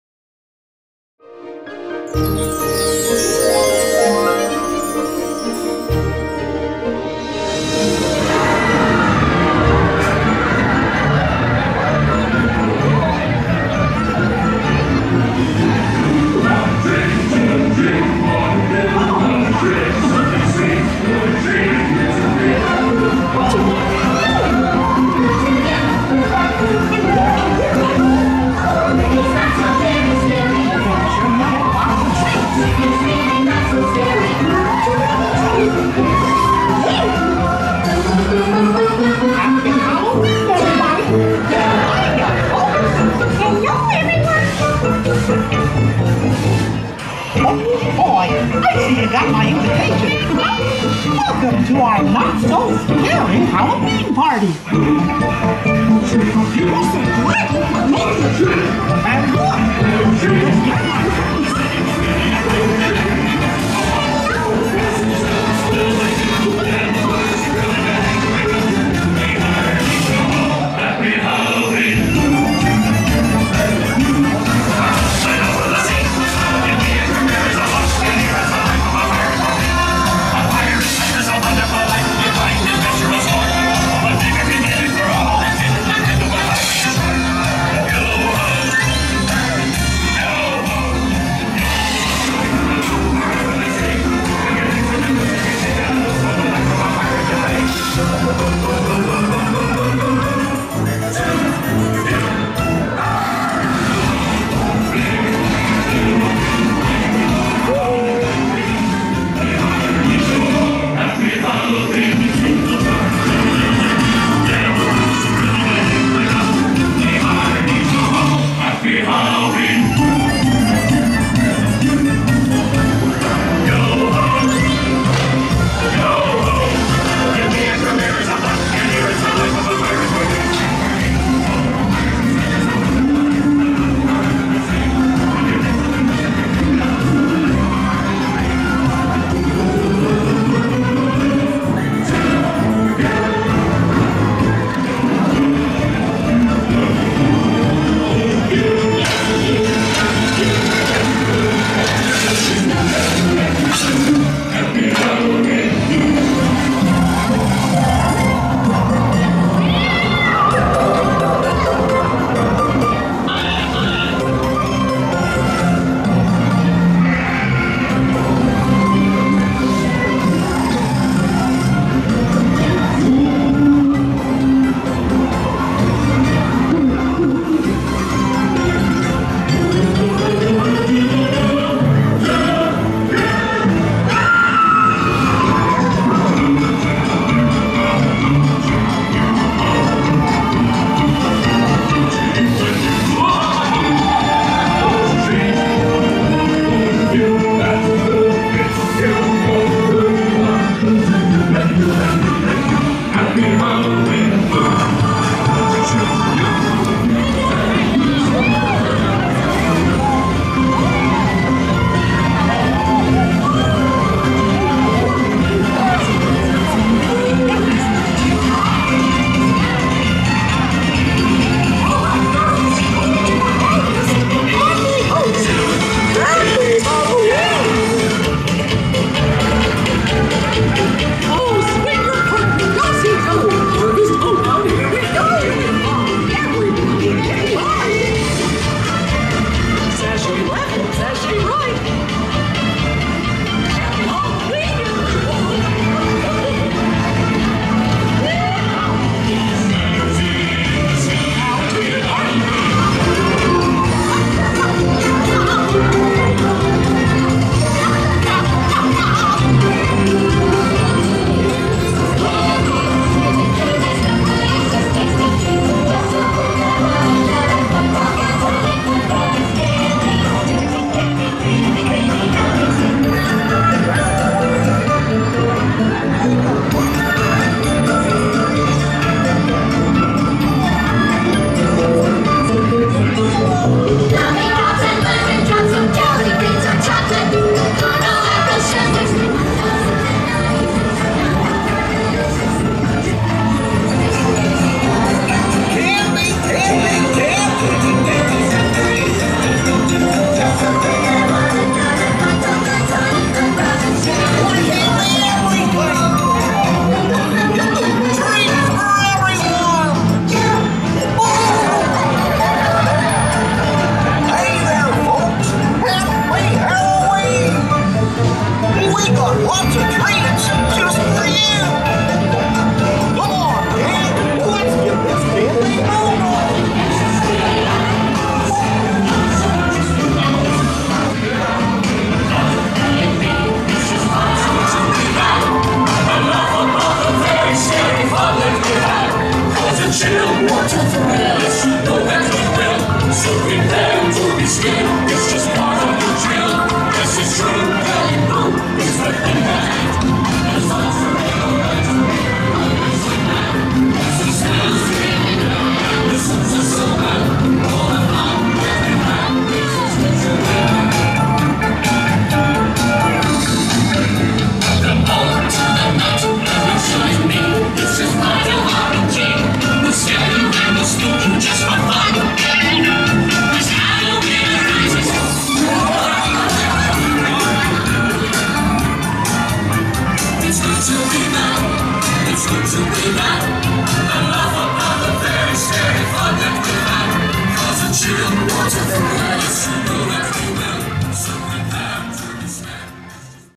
BOO TO YOU Parade 2014 - Mickey's Not So Scary Halloween Party
With a few changes for 2014, here is the Mickey's Not So Scary Halloween Party BOO TO YOU Parade!